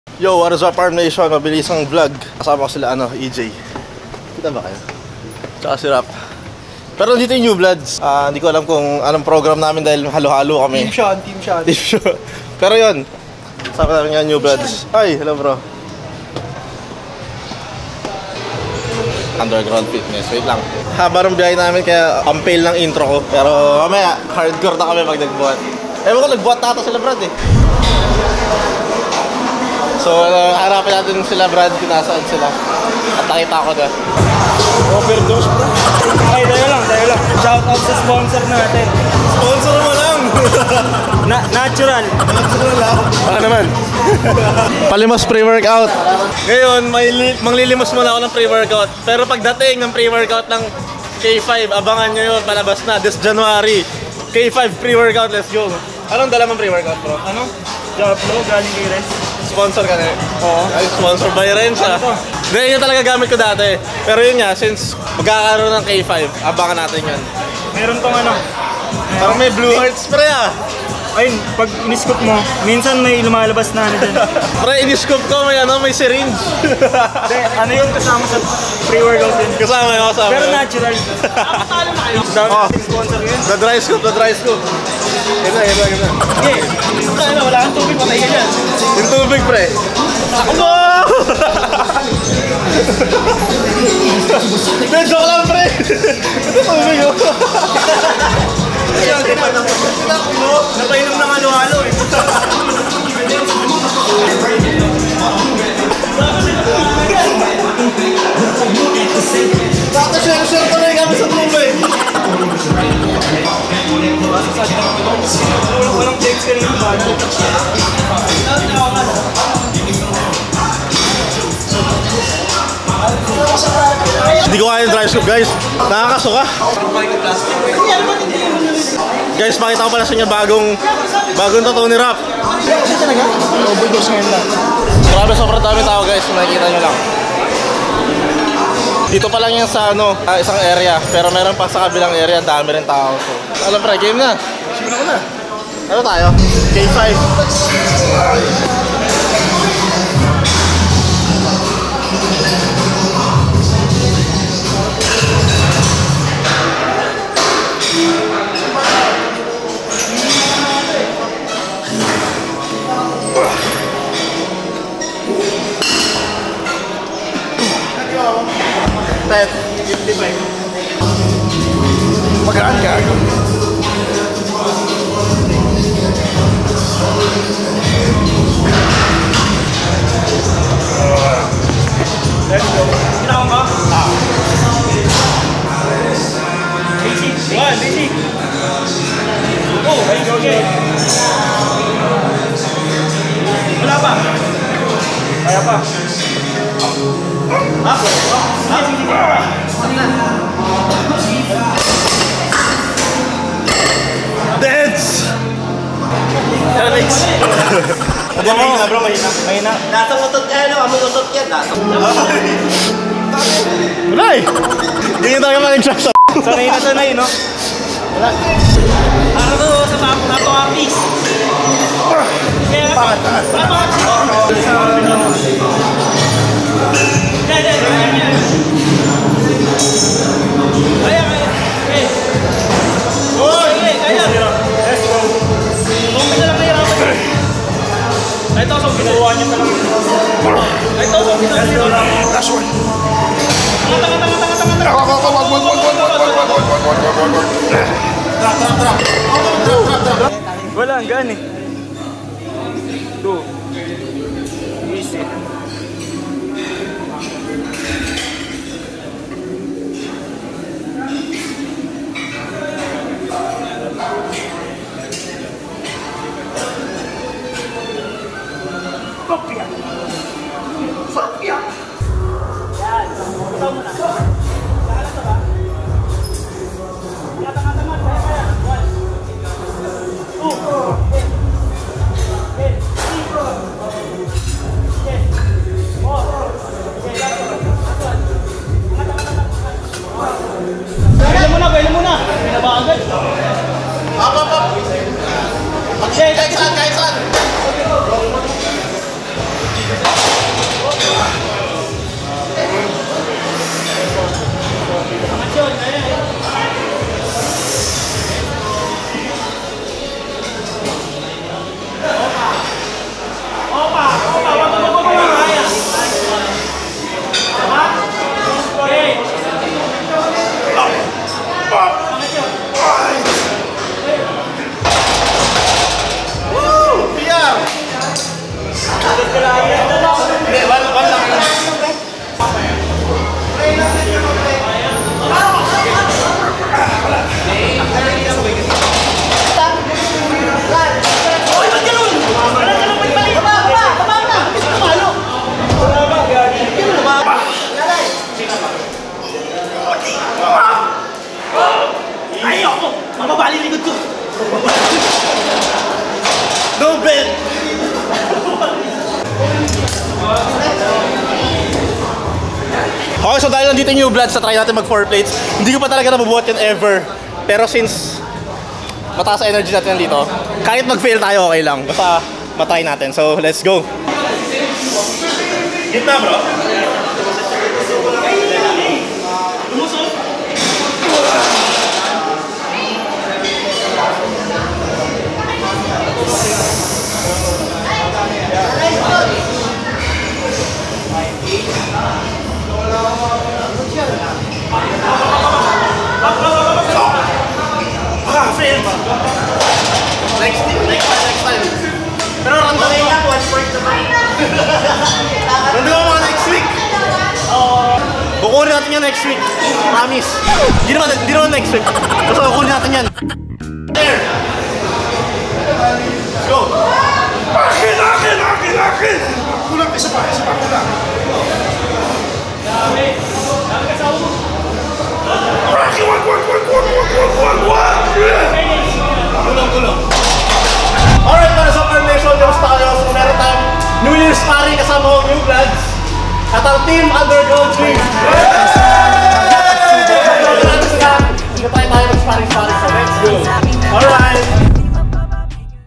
1st Team Training ng 2024!
Nag dala kami ulit ng Table sa Underground Fitness Gym! At syempre dahil nag sama sama nanaman kami, heavy lifting nanaman to matik!